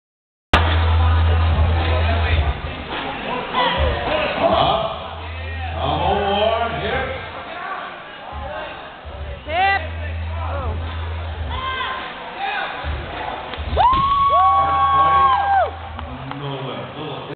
RawUnity - 315lb deadlift - FAIL hitching - Ginger Vieira 148lb Raw Female
So close! Truly enjoyed trying!